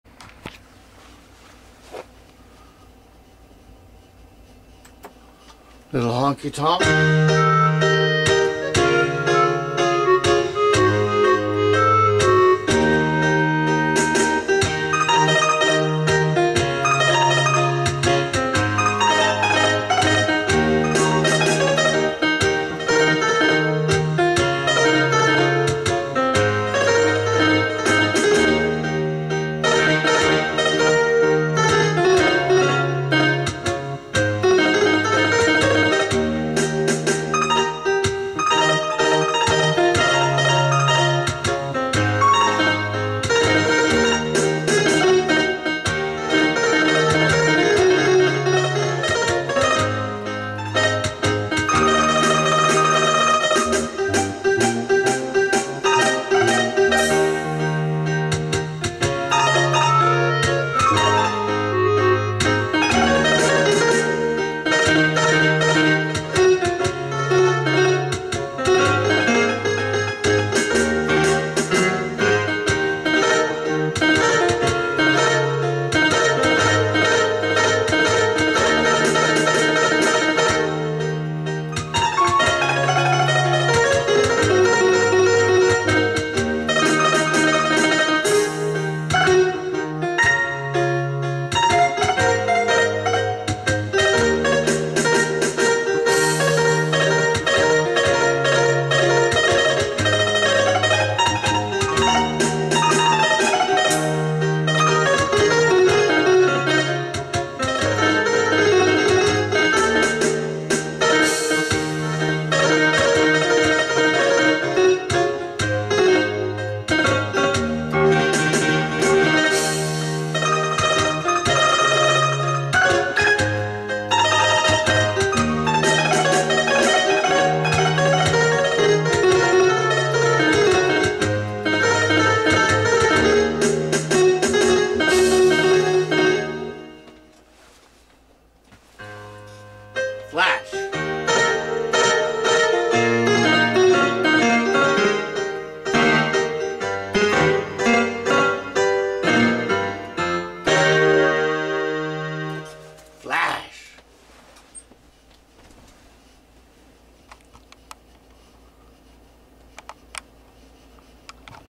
dirty old honky cat flasher
This is a little comical video I did with one of my compositions. It is kind of funny and it is OK for kids--The music is great.